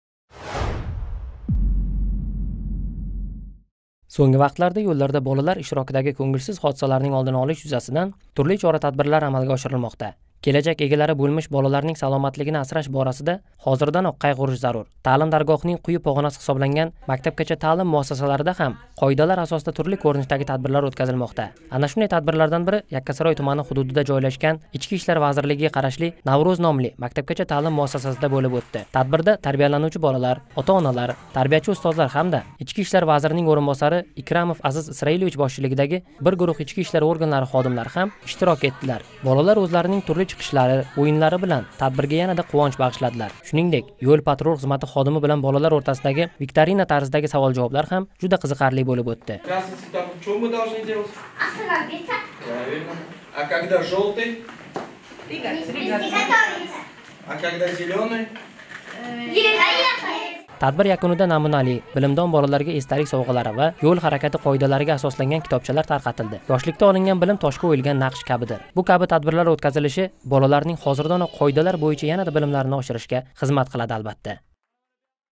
Yoshlikda olingan bilim — toshga o‘yilgan naqsh kabidir
Poytaxtdagi "Navro‘z" nomli MTMda qoidalar yuzasidan tashkil etilgan tadbir savol-javob va o‘yinlar tarzida juda qiziqarli bo‘lib o‘tdi.